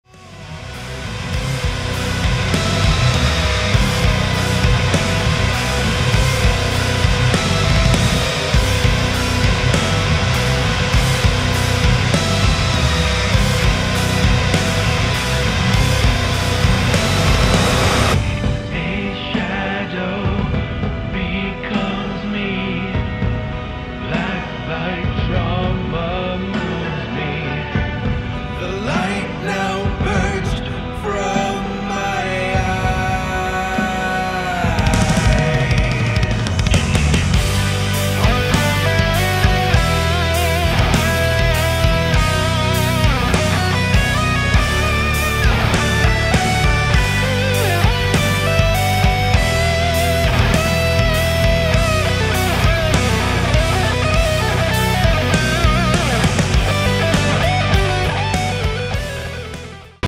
Album 3 Sneak Preview feat The Slovenian Octopus (Klemen Markelj)
Some early mix work in progress for you to check out. Klemen is always fun to watch so I figure a few of you guys would enjoy this clip.